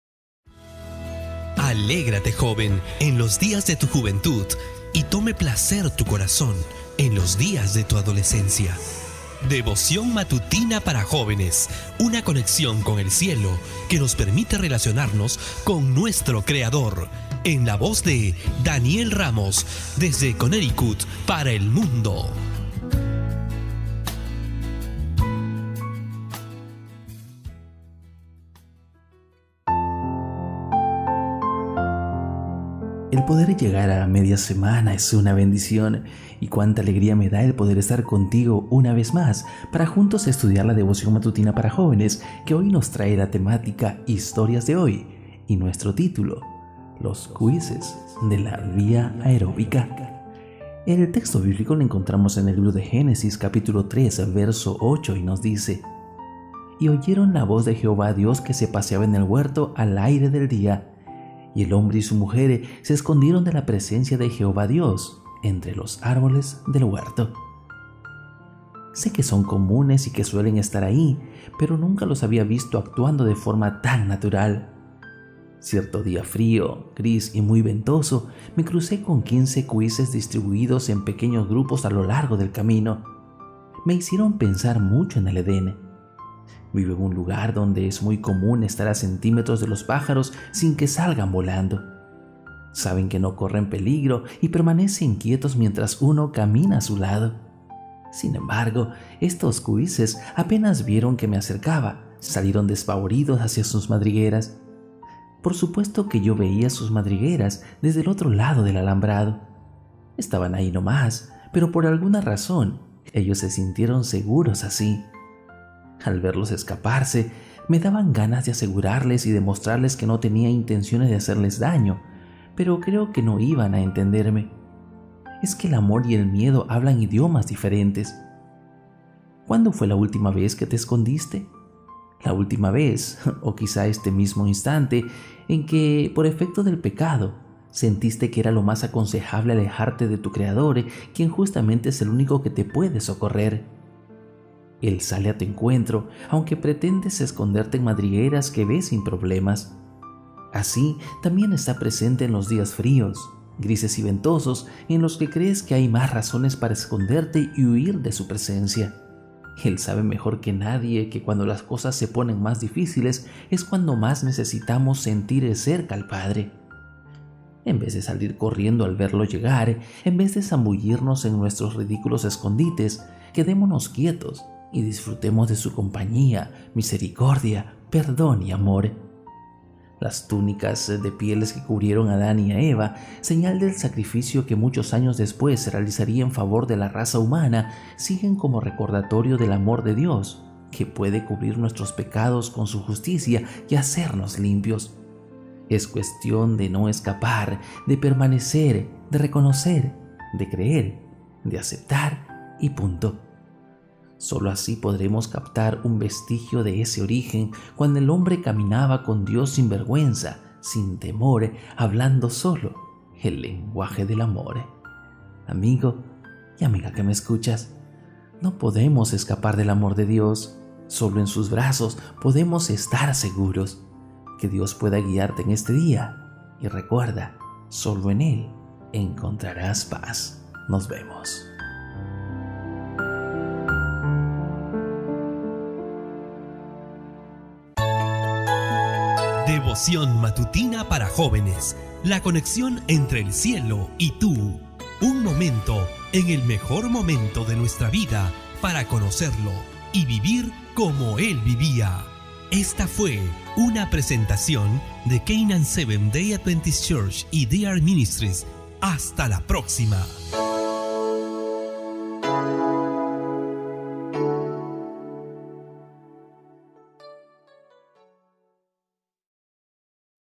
Devoción Matutina Para Jóvenes - 21 de Julio del 2021
HISTORIAS DE HOY

LOS CUISES DE LA VÍA AERÓBICA

“Y oyeron la voz de Jehová Dios que se paseaba en el huerto, al aire del día; y el hombre y su mujer se escondieron de la presencia de Jehová Dios entre los árboles del huerto ”(Gén. 3: 8).

Sé que son comunes y que suelen estar ahí, pero nunca los había visto actuando de forma tan natural.

Cierto día frío, gris y muy ventoso, me crucé con quince cuises distribuidos en pequeños grupos a lo largo del camino. Me hicieron pensar mucho en el Edén.

Vivo en un lugar donde es muy común estar a centímetros de los pájaros sin que salgan volando. Saben que no corren peligro y permanecen quietos mientras uno camina a su lado. Sin embargo, estos cuises, apenas vieron que me acercaba, salieron despavoridos hacia sus madrigueras. Por supuesto que yo veía sus madrigueras desde el otro lado del alambrado. Estaban ahí no más, pero por alguna razón ellos se sintieron seguros así.

Al verlos escaparse, me daban ganas de asegurarles y demostrarles que no tenía intenciones de hacerles daño, pero creo que no iban a entenderme. Es que el amor y el miedo hablan idiomas tan diferentes ...

¿Cuándo fue la última vez que te escondiste? La última vez (o quizás este mismo instante) en que, por efecto del pecado, sentiste que era lo más aconsejable alejarte de tu Creador, quien justamente es el único que te puede socorrer.

Él sale a tu encuentro, aunque pretendas esconderte en “madrigueras” que ve sin problemas. Y sí, también está presente en los días fríos, grises y ventosos en los que crees que hay más razones para esconderte y huir de su presencia.

Él sabe mejor que nadie que, cuando las cosas se ponen más difíciles, es cuando más necesitamos sentir cerca al Padre.

En vez de salir corriendo al verlo llegar, en vez de zambullirnos en nuestros ridículos escondites, quedémonos quietos y disfrutemos de su compañía, misericordia, perdón y amor.

Las túnicas de pieles que cubrieron a Adán ya Eva, señal del sacrificio que muchos años después se realizóía en favor de la raza humana, siguen como recordatorio del amor de Dios que puede cubrir nuestros pecados con su justicia y hacernos limpios. Es cuestión de no escapar, de permanecer, de reconocer, de creer, de aceptar. Y punto.

Solo así podremos captar un vestigio de ese origen, cuando el hombre caminaba con Dios sin vergüenza, sin temor, hablando solo el lenguaje del amor.

HOY CAMINO CON DIOS
Devoción Matutina para Jóvenes 2021
Narrado por: Daniel Ramos
Desde: Connecticut, Estados Unidos